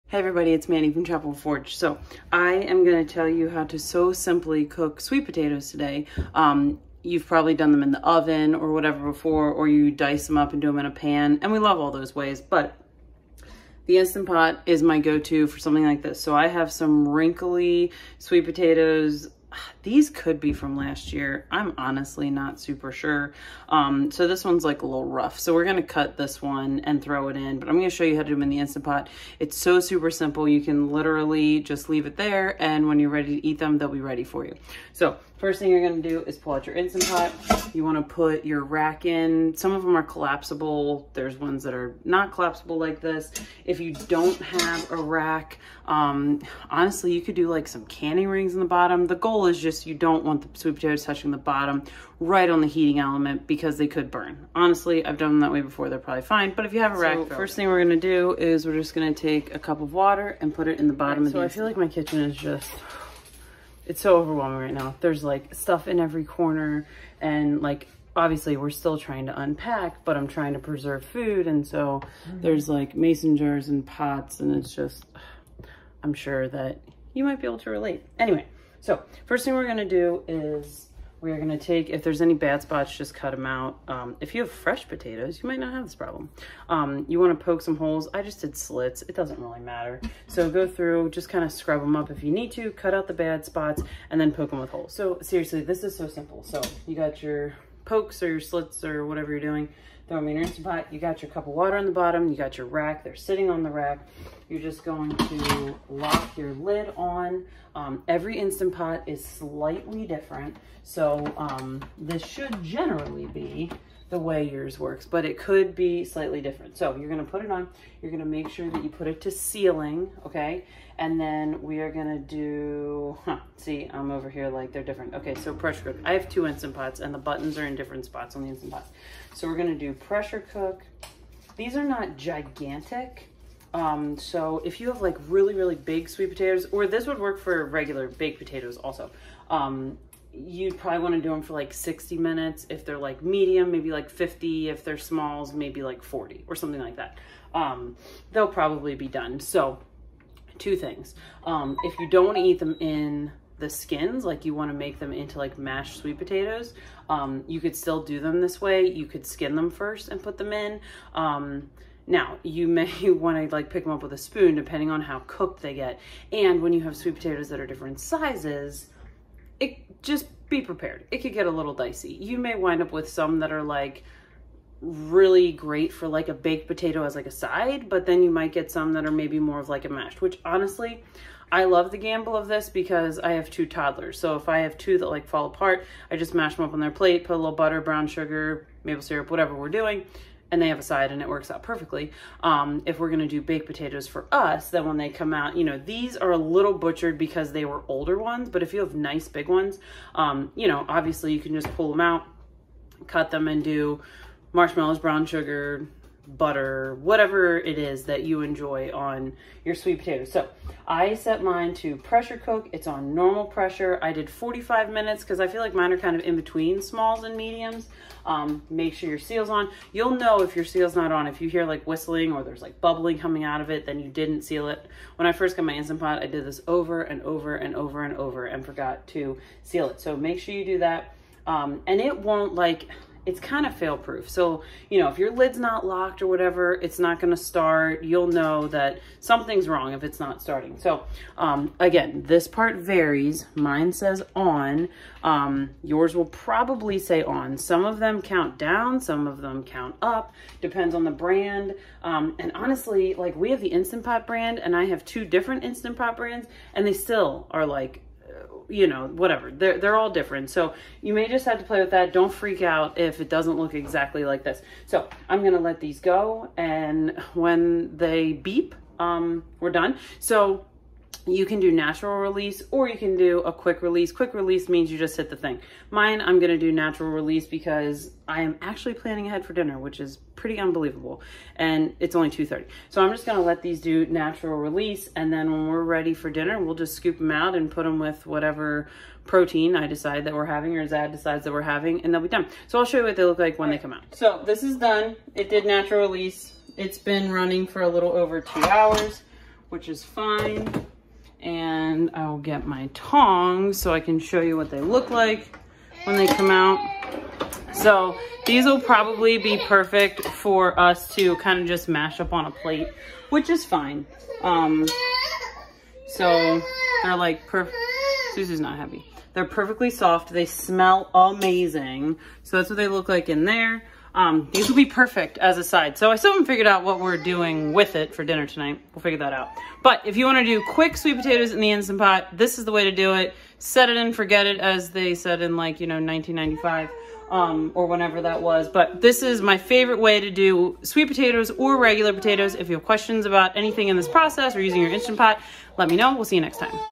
How to Cook Sweet Potatoes in the Instant Pot - Easy Mashed Sweet Potatoes
Quick and Easy Instant Pot Sweet Potatoes Recipe - Perfectly Cooked Every Time

It's so easy to cook delicious sweet potatoes in the Instant Pot! If you've ever wondered how to achieve that perfect, tender sweet potato without the fuss, this process is for you. Whether you're a busy parent, a college student, or just looking for a hassle-free way to prepare this nutritious root vegetable, we've got you covered.

In this video, we'll walk you through the step-by-step process of cooking sweet potatoes in the Instant Pot. You'll be amazed at how quickly you can whip up a nutritious side dish or the base for countless sweet and savory recipes.

Recipe:
Place rack in Instant Pot. Add 1 cup of water. Place sweet potatoes in. Cook 40 mins for small, 50 mins for medium, and 60 for large. Simply lock the lid, hit Pressure Cook, and set time accordingly. Don't forget to close the vent. They will be delicious!

Here's what you can expect to learn:
🍠 Prepping your sweet potatoes for the Instant Pot.
🍠 Setting up your Instant Pot for the perfect cooking environment.
🍠 Cooking time and pressure settings for different sweet potato sizes.
🍠 Quick release vs. natural release – which one to choose?

Our Instant Pot sweet potato recipe is a game-changer because it:
✅ Reduces cooking time dramatically compared to conventional methods.
✅ Preserves the sweet potato's natural flavor and nutrients.

So, whether you're planning to enjoy them as a simple side dish with butter and a sprinkle of cinnamon or incorporate them into your favorite recipes like sweet potato fries, pies, or mash, this Instant Pot method will become your go-to.

Ready to make your life in the kitchen easier and tastier? Let's dive in and cook up some incredible Instant Pot sweet potatoes together. 🍠👩‍🍳👨‍🍳

Learn more about food preservation and recipes to use your food stores on our YouTube channel and our website blog